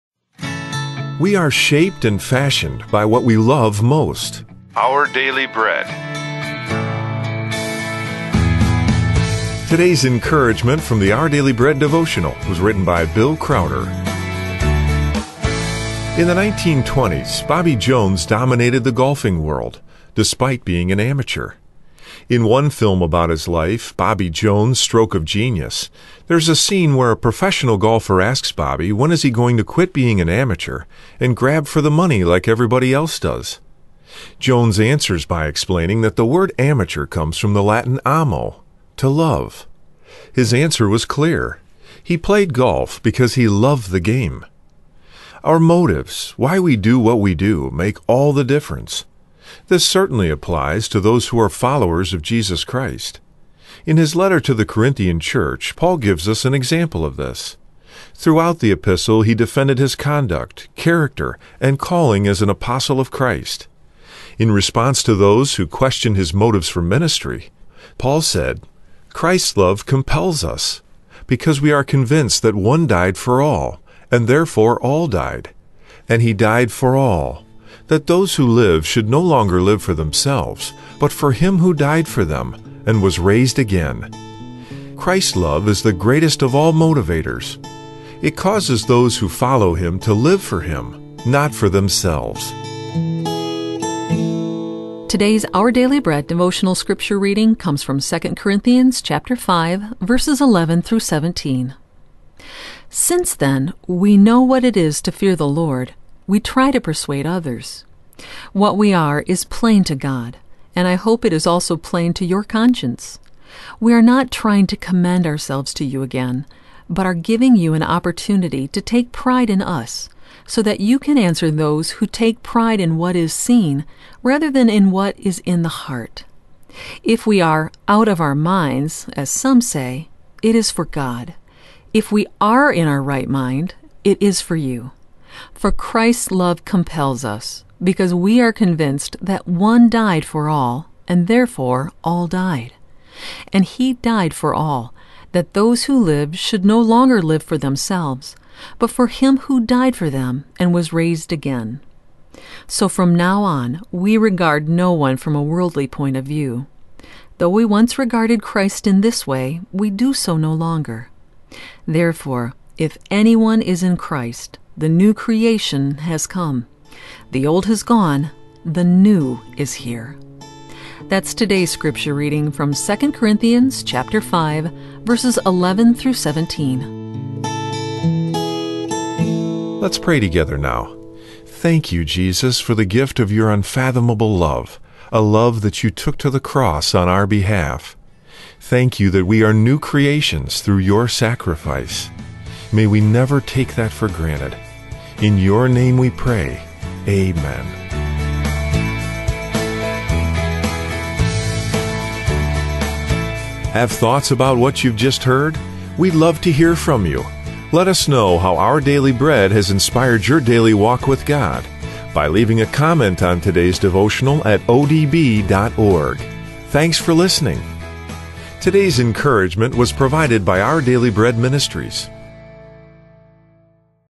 MOTIVATED BY LOVE by Bill Crowder
We are shaped and fashioned by what we love most.

In the 1920s, Bobby Jones dominated the golfing world, despite being an amateur. In one film about his life, Bobby Jones: Stroke of Genius, there is a scene where a professional golfer asks Bobby when he is going to quit being an amateur and grab for the money like everyone else does. Jones answers by explaining that the word amateur comes from the Latin amo—to love. His answer was clear: He played golf because he loved the game.

Our motives, why we do what we do, make all the difference. This certainly applies to those who are followers of Jesus Christ. In his letter to the Corinthian church, Paul gives us an example of this. Throughout the epistle he defended his conduct, character, and calling as an apostle of Christ. In response to those who questioned his motives for ministry, Paul said, “Christ’s love compels us, because we are convinced that one died for all, and therefore all died. And he died for all, that those who live should no longer live for themselves but for him who died for them and was raised again” (2 Cor. 5:14-15 niv).

Christ’s love is the greatest of all motivators. It causes those who follow Him to live for Him, not for themselves.

Read: 2 Corinthians 5:11–17

The love of Christ compels us. —2 Corinthians 5:14

What are some of the ways your understanding of Christ and His love has shaped your motives and your actions? In what ways would you like to see God work in you now?

Insight:

In 2 Corinthians Paul was writing to a church that he founded—yet a church that had, in a sense, turned on him. This makes it very different from 1 Thessalonians, where Paul was writing to men and women with whom he had a strong and loving relationship. Because of these different relationships, in 1 Thessalonians 2–3 Paul was describing his ministry while in 2 Corinthians was defending it.